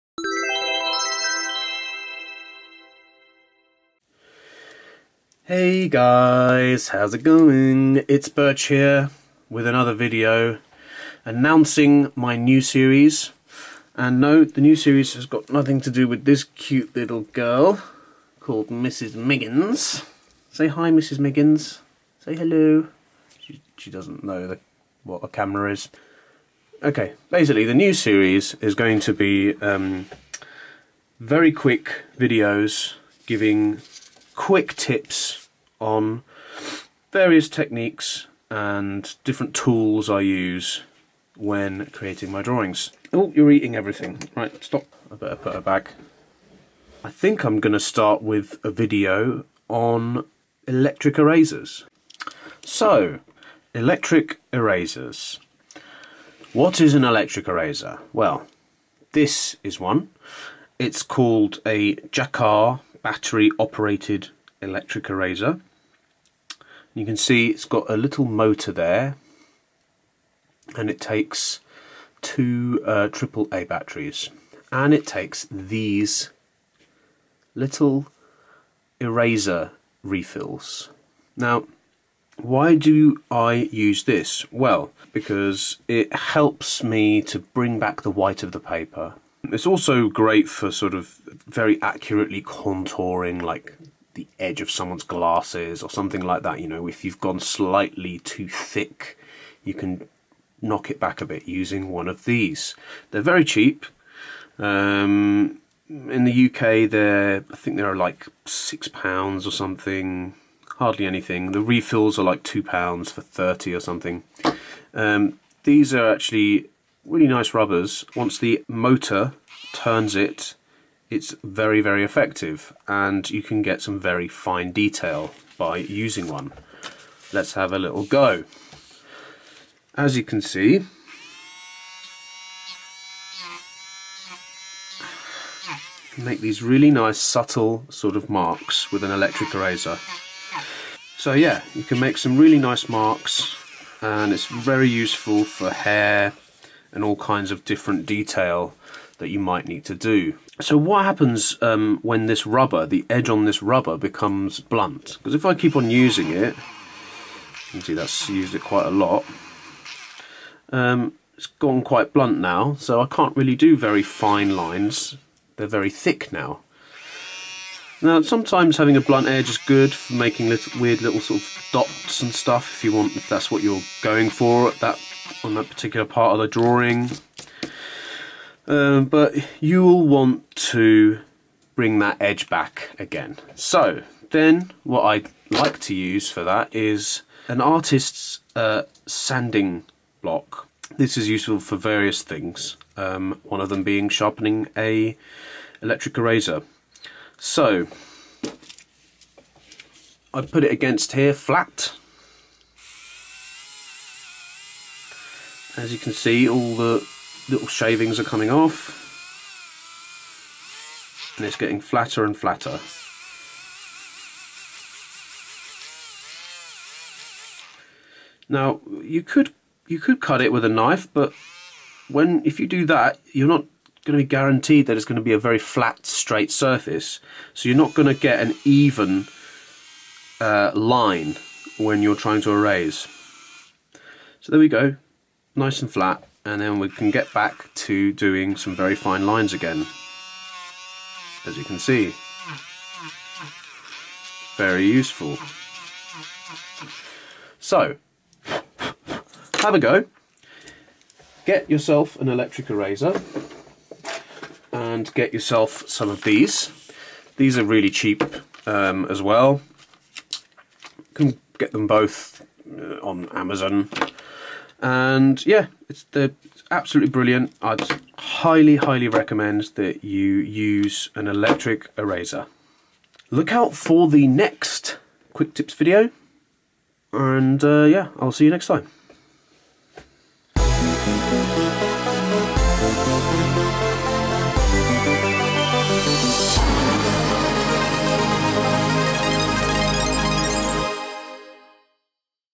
New Series! Quick Tip #1 - The Electric Eraser
Tools/Pencils/Paper I used:

Caran D'ache Luminance coloured pencils
Faber Castell Polychromos coloured pencils
Jakar electric eraser
Tombow mono zero eraser
Strathmore 500 series Bristol Plate 14" x 17"

Filmed using a GoPro hero 4 silver

Photo stills taken on a canon 550D

Tutorials filmed with a Sony RX100 mark i